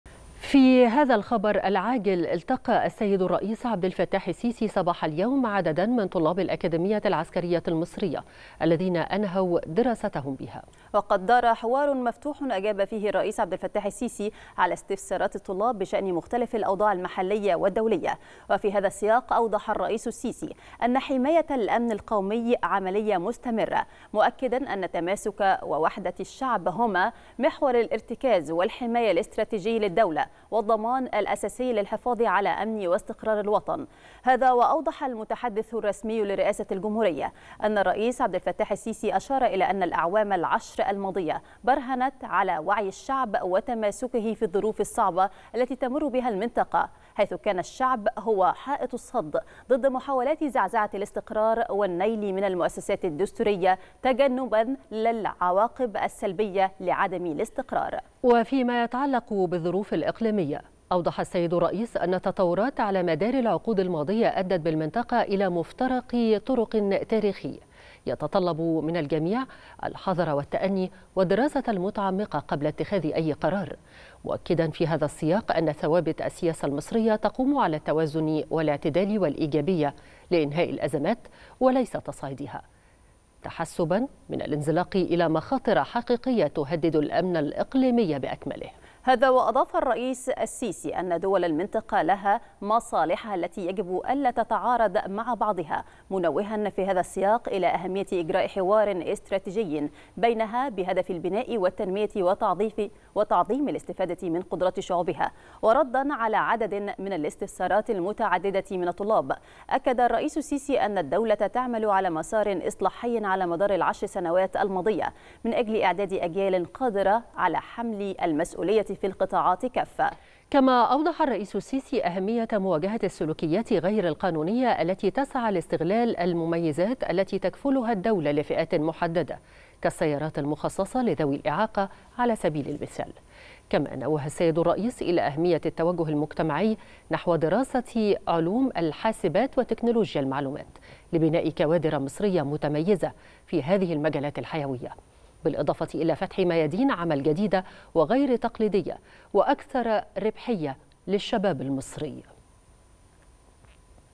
الرئيس السيسي التقى صباح اليوم مع عدد من طلاب الأكاديمية العسكرية المصرية الذين أنهوا دراستهم بها
نشرة السادسة | متحدث الرئاسة: الرئيس السيسي التقى صباح اليوم مع عدد من طلاب الأكاديمية العسكرية المصرية الذين أنهوا دراستهم بها